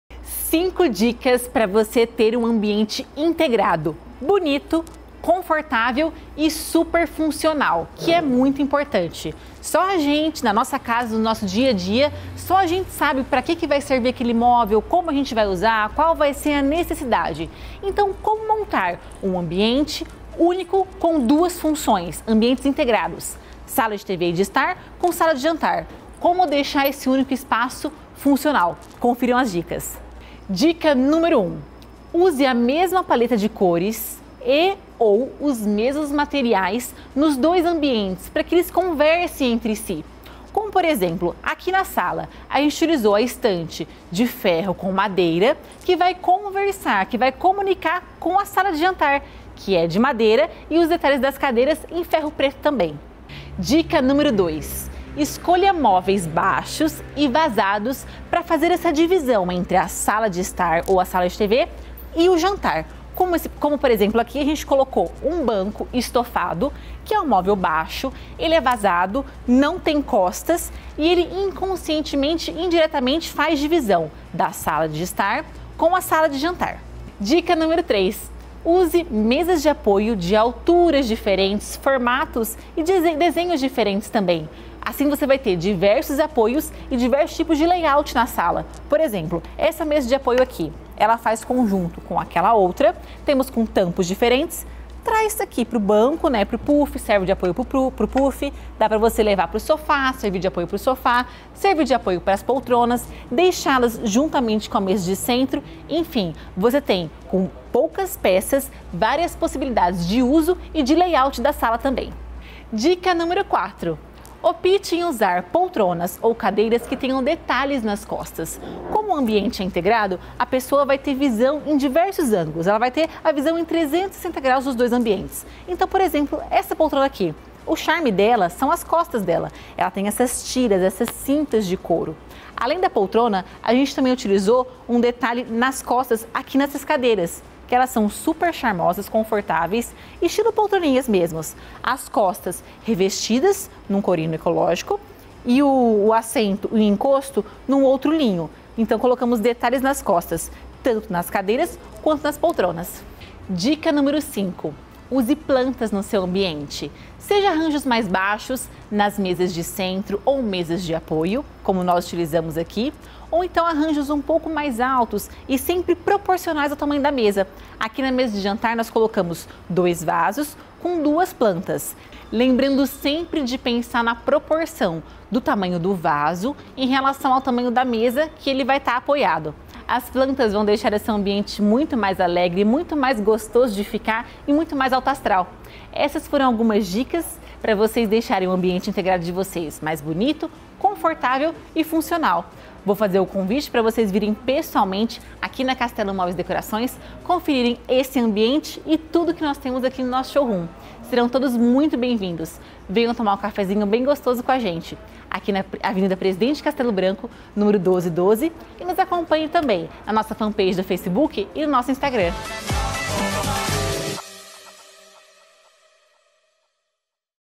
COMO MONTAR UM AMBIENTE INGRADO BONITO E FUNCIONAL? | LELINHA GENTIL
Quem aí mora numa casa / apartamento com ambientes integrados?!
Num espaço único, sem paredes, tem a sala de jantar com o Home ou com o living!
Confiram 5 dicas da nossa designer de Interiores, Maibi Tasqueti, para você ter um ambiente integrado, bonito, confortável e super funcional! ✨😍😍

Conhece alguém que é 🥰apaixonado por decoração?? Então envie esse vídeo para ele!